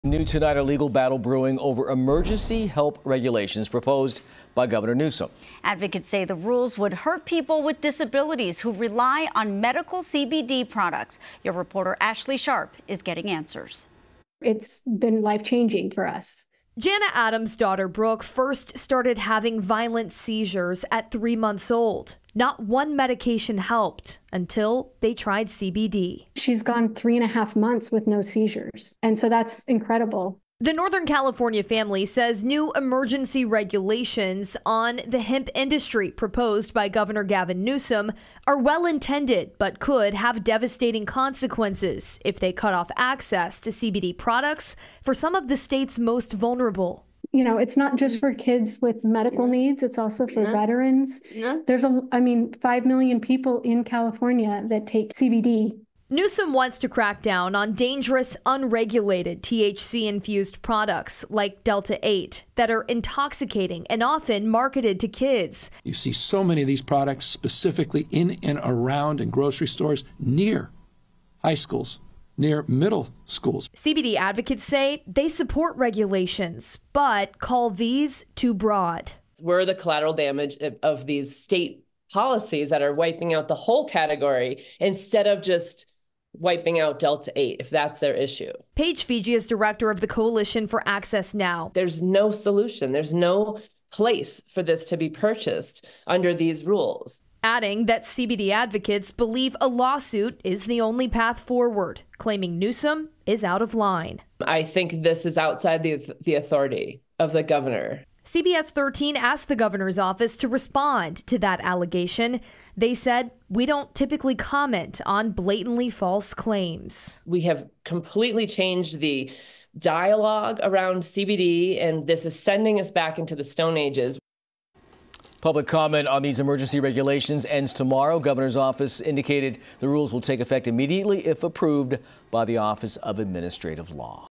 Could hemp industry advocates sue California governor?
Could Gov. Gavin Newsom be sued by hemp industry advocates? The group One Hemp says they're considering filing a lawsuit if emergency hemp regulations proposed by the governor earlier this month are passed. Advocates say the regulations hurt people with disabilities who rely on medical CBD products.